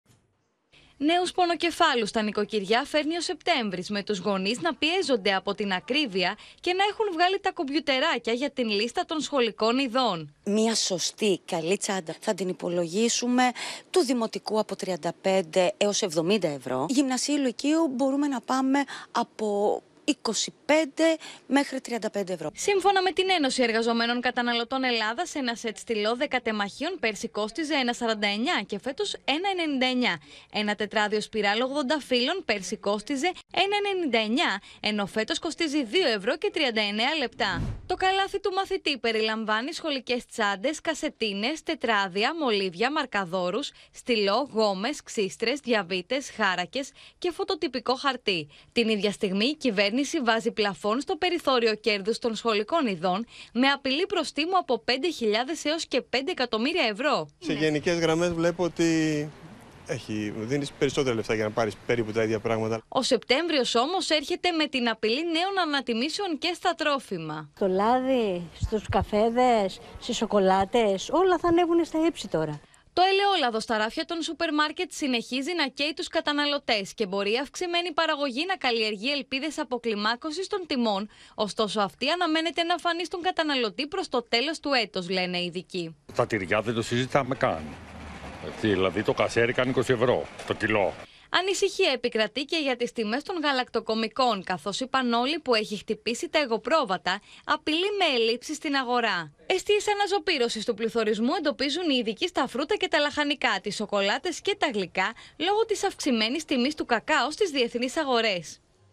Ακρίβεια: Ακόμα πιο βαρύς ο λογαριασμός με την αγορά των σχολικών ειδών | OPEN TV
Βαθιά το χέρι στη τσέπη αναμένεται να βάλουν τα νοικοκυριά και αυτόν τον Σεπτέμβριο για να γεμίσουν τις σχολικές τσάντες των παιδιών με τα απαραίτητα. Αύριο ξεκινάει το καλάθι του μαθητή το οποίο θα περιλαμβάνει βασικά σχολικά είδη. Μαζί με τα σχολικά έξοδα, υπάρχει και ο φόβος νέων ανατιμήσεων στα τρόφιμα.